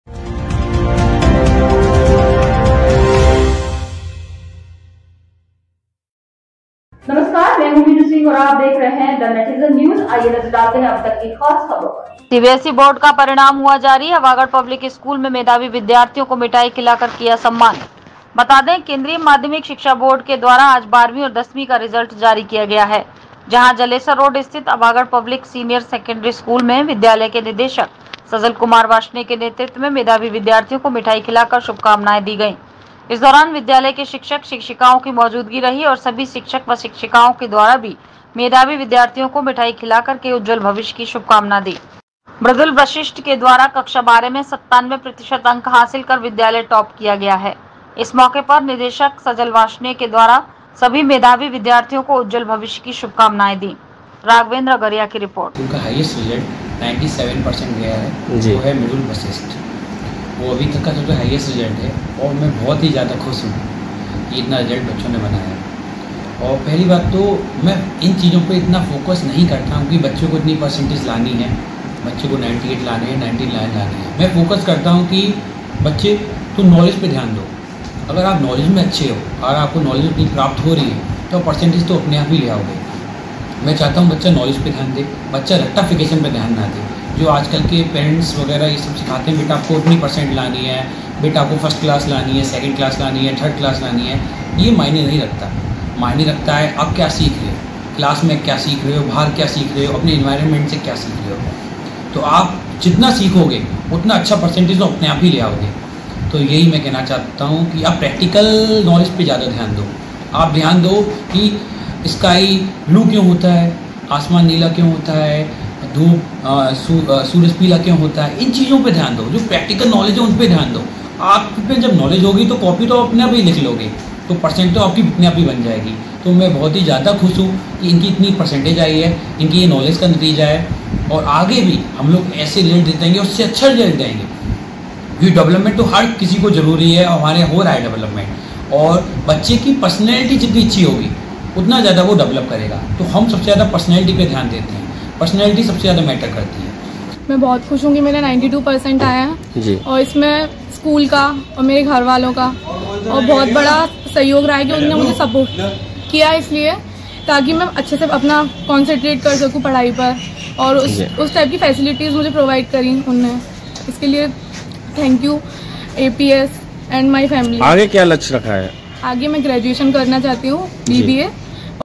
सीबीएसई बोर्ड का परिणाम हुआ जारी
अवागढ़ पब्लिक स्कूल में मेधावी विद्यार्थियों को मिठाई खिलाकर किया सम्मान
 बतादे केंद्रीय माध्यमिक शिक्षा बोर्ड के द्वारा आज 12वीं एवं दसवीं का रिजल्ट जारी किया गया है